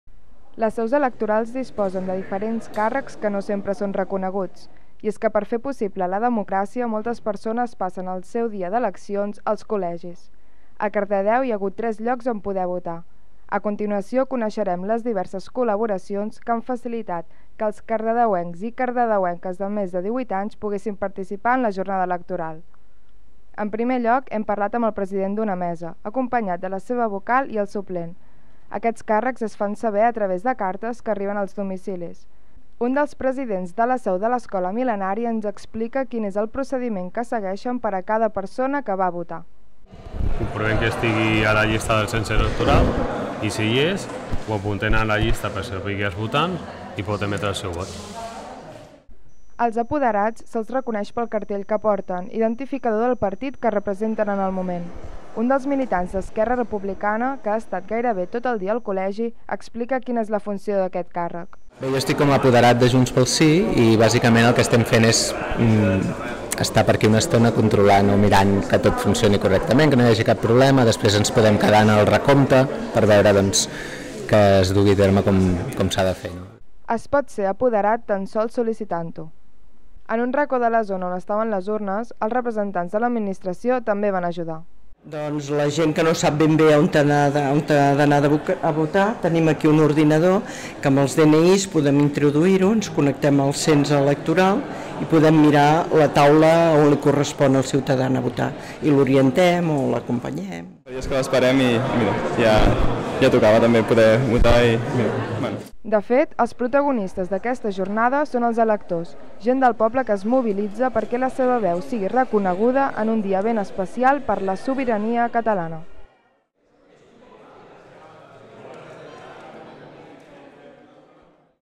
Com funciona un col·legi electoral?
El darrer 27 de setembre ha servit també per aprendre com funciona una seu electoral. El col·legi Mil·lenari reuneix presidents, vocals, suplents de mesa, representats de l'administració, apoderatsi electors i electores, per suposat!